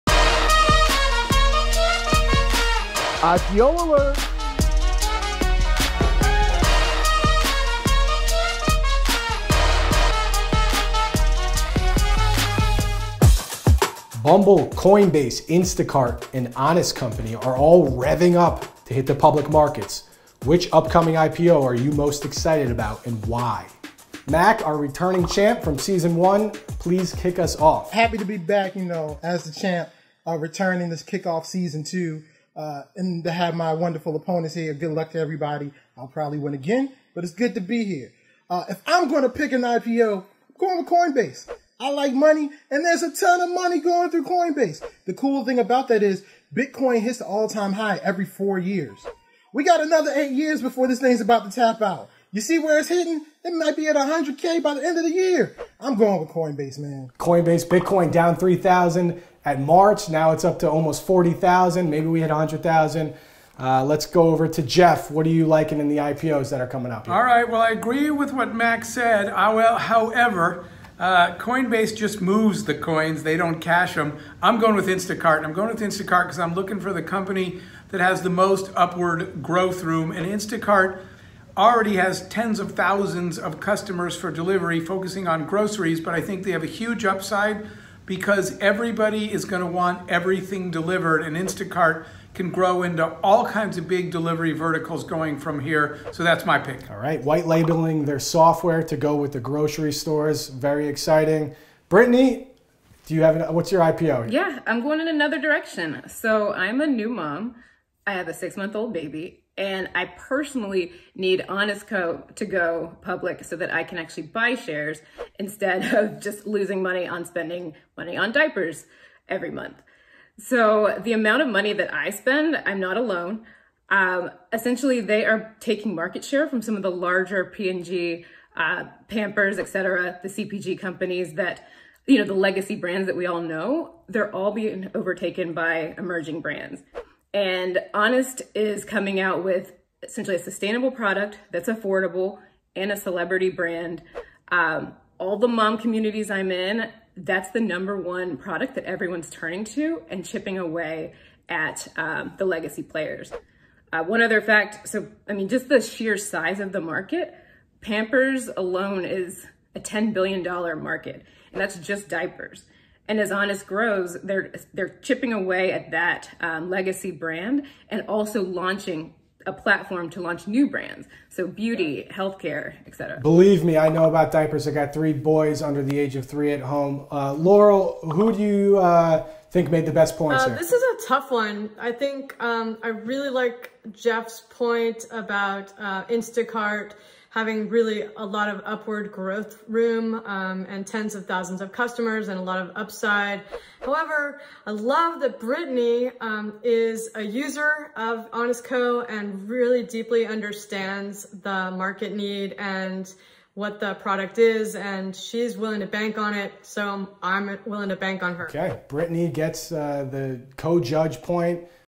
Should You Invest in Coinbase, Instacart and Honest Company?
PTVC Clips! IPO Alert segment from Episode 9.